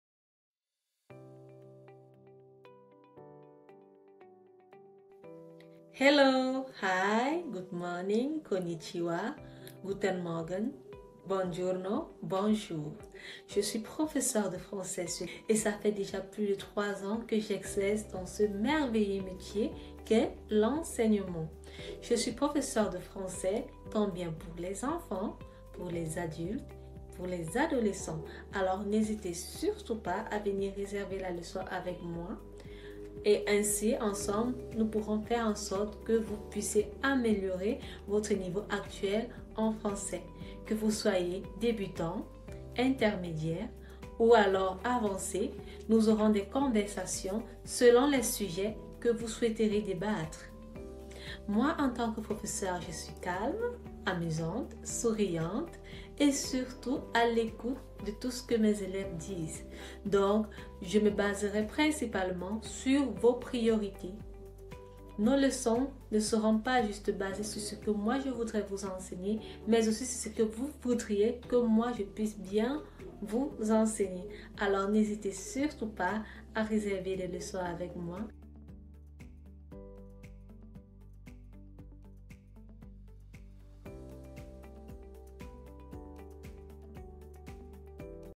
Certified French Teacher For A1-C2 in Segulung Kec. Dagangan, Kabupaten Madiun for Fr... - Asta S.
Hello I am Asta. A french online tutor. I have been teaching french for over 3 years now. My teaching style is very simple. I focus on the student's goals. If you want a person to improve your french level either on conversation, grammar, conjugation or simply prepare yourself for exams I will be there to help you. Don't hesitate to book lessons with me